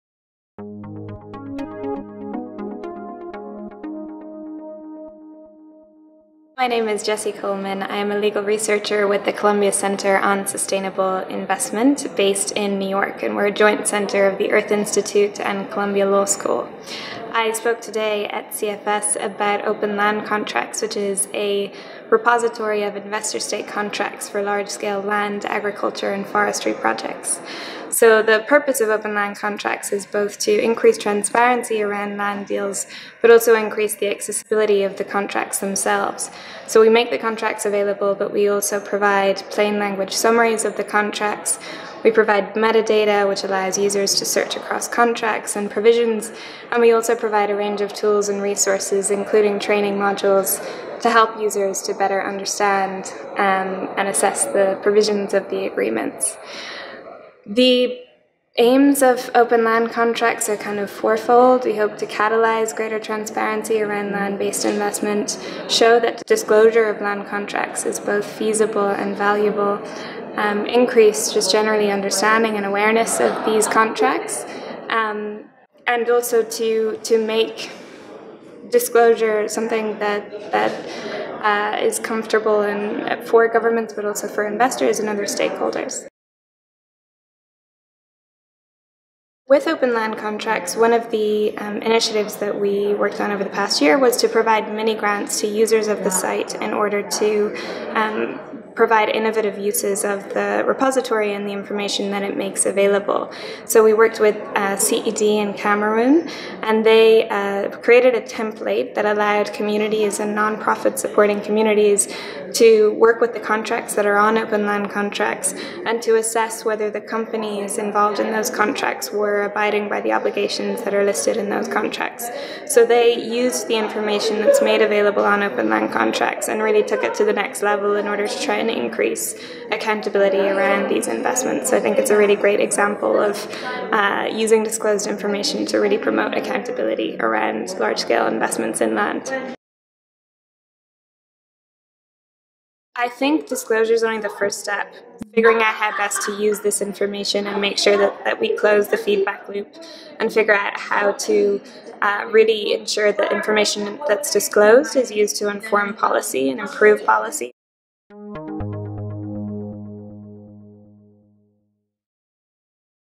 Jesse Coleman, CCSI on the open land contracts online repository
Jesse Coleman participated in the side-event organised by the Global Donor Working Group on Land during CFS44. She works at the Columbia center on sustainable investments (CCSI) in New York and she spoke about the Open land contracts repository of the CCSI. The online repository serves to increase transparency of domestic and international investments in land, supporting local administrations, farmers and private sector with developing templates and gathering information.